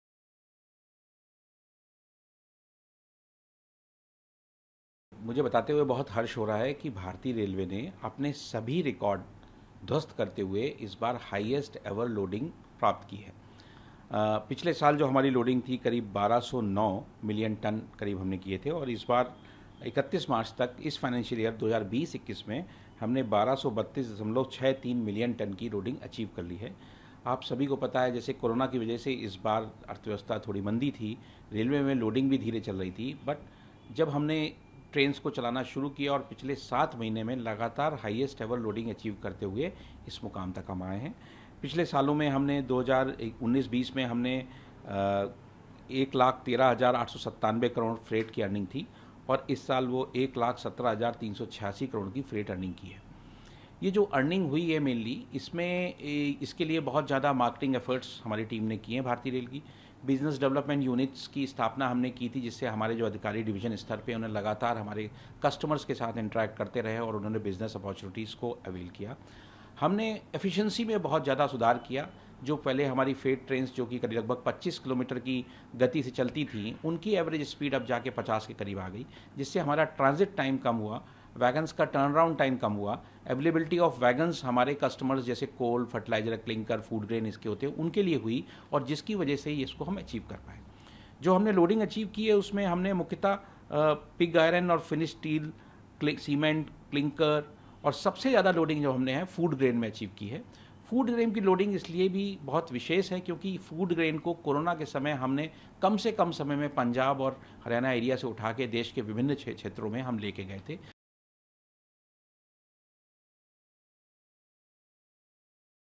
Indian Railways breaks all records, achieves highest loading in FY 2020-21
New Delhi, Apr 01 (ANI): Director (Information & Publicity) of Railway Board RD Bajpai expressed his happiness over breakage of all previous records and said he is grateful that the railways had achieved highest ever loading breaking all the records. “Indian Railways had achieved highest ever loading breaking all records. In the financial year of 2020-21, total loading was 1232.63 million tonnes while it was 1209.32 in financial year 2019-20, which is 1.93% higher compared to last year’s loading for the same period. Previous year, the earning was 1,13,897 crore but this year it is 1,17,386 crore,” Bajpai said. “Business Development Units were established, regular interaction with customers to avail the business, improvement in efficiency, reduction of transit time were the efforts made by our team. Food grain, cement, and other materials were loaded. Mainly food grains had been very important as we had supplied it within a short span of time to Punjab and Haryana areas during coronavirus pandemic,” he added.